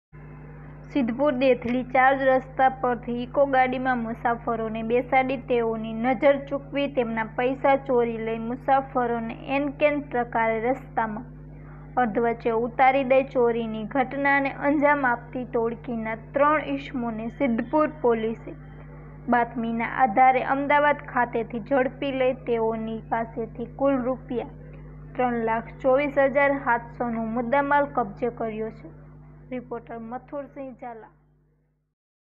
સિદ્ધપુર માં ચોરીની ઘટનાંને અંજામ આપતી ટોળકીનાં ત્રણ ઈસમોને ઝડપી પડ્યા
સિદ્ધપુર દેથળી ચાર રસ્તા પરથી ઈકો ગાડીમાં મુસાફરોને બેસાડી તેઅોની નજર ચુકવી તેમનાં પૈસા ચોરી લઈ મુસાફરોને અેનકેન પ્રકારે રસ્તામાં અધવચ્ચે ઉતારી દઈ ચોરીની ઘટનાંને અંજામ આપતી ટોળકીનાં ત્રણ ઈસમોને સિદ્ધપુર પોલીસે બાતમીનાં આધારે અમદાવાદ ખાતેથી ઝડપી લઇ તેઅોની પાસેથી કુલ રૂપિયા ૩૨,૪૭૦૦નો મુદ્દામાલ કબ્જે કર્યો છે.

પાટણ જીલ્લાનાં સિદ્ધપુર હાઈવે પર દેથળી ચાર રસ્તાથી ખાનગી ઈકો ગાડીમાં મુસાફરોને બેસાડી તેમની  પાસેનાં સરસામાનમાંથી સિફ્તા પૂર્વક પૈસા સેરવી લેવાની ઘટનાં છેલ્લા કેટલાક મહિનાઅોથી પ્રકાશમાં આવવા પામી હતી જેમાં સિદ્ધપુર તાલુકાના કાલોડા ગામનું દંપતી પાક ધિરાણનાં રૂપિયા ૨,૩૪૦૦૦ સિદ્ધપુરની અેસબીઆઈમાં બેંકમાં ભરવા માટે આવ્યા હતાં પરંતુ પૈસા ભરવાનો સમય પુરો થઈ ગયો હોવાથી બેંક કર્મચારીઓ અે બીજા દિવસે આવવાનું કહ્યું હતું જેથી આ દંપતી કમલીવાડા જવા માટે  દેથળી ચાર રસ્તાથી અેક યયભજ્ઞ ગાડીમાં બેઠુ હતુ જેમાં પહેલેથી જ અન્ય મુસાફરો બેઠેલા હતા જેમાંથી ચોરીને અંજામ આપતી ટોળકીનાં માણસોઅે સિફ્તા પૂર્વક પૈસા ભરેલી થેલીને ચેકો મારી ૨,૩૪ લાખ લઈ લીધા હતા ત્યારે ગાડી ચાલકે ગાડીની બ્રેક ફેલ થઈ ગઈ હોવાનું જણાવી દંપતિને અધવચ્ચે રસ્તામાં ઉતારી દઈ ગાડી લઈ ફરાર થઈ ગયા હતા ત્યારે બીજા અેક બનાવમાં સિદ્ધપુર દેથળી ચાર રસ્તાથી પેસેન્જરને  ઈકો ગાડીમાં બેસાડી તેના થેલામાંથી પણ રૂપિયા ૩૬,૦૦૦ની ચોરી કરી તેને પણ અધવચ્ચે ગાડીમાંથી ઉતારી દેવામાં આવ્યો હતો જે બનાવ અંગે સિદ્ધપુર પોલીસ મથકે ફરીયાદ દાખલ થવા પામી છે ત્યારે ચોરીની આ ઘટનાંઅોનો ભેદ ઉકેલવા માટે સિદ્ધપુર પી.આઈ. ચિરાગ ગોસાઈઅે અલગ અલગ પોલીસ ટીમો બનાવી તપાસના ચક્રોગતિમાન કર્યા છે સિદ્ધપુર પોલીસ પેટ્રોલિંગમાં હતી તે દરમ્યાન ખાનગી રાહે બાતમી મળી હતી કે સિદ્ધપુર દેથળી ચાર રસ્તાથી મુસાફરોને ઈકો ગાડીમાં બેસાડી પૈસાની ચોરી કરનાર ગેંગનાં ત્રણ ઈસમો અમદાવાદ ખાતે છે ત્યારે પોલીસે અમદાવાદ ખાતે જઈને આ ત્રણેય આરોપીઅોની ધરપકડ કરી તેઅોની સધન પુછપરછ કરતાં તેઅોઅે ગુનાની કબુલાત કરતાં પોલીસે તેમની પાસેથી રોકડ ૨૦,૨૦૦ મોબાઇલ નંગ- ૨ કિંમત રૂપિયા ૪૫૦૦ તેમજ ઈકો ગાડી કિંમત રૂપિયા ૩ લાખ મળી કુલ રૂપિયા ૩,૨૪,૭૦૦નો મુદ્દામાલ કબ્જે કરી વધુ આગળની તપાસ હાથ ધરી છે તેવું અત્રેના સુત્રો પાસેથી હકીકતો જાણવા મળી હતી.

રિપોર્ટર  મથુરસિંહ ઝાલા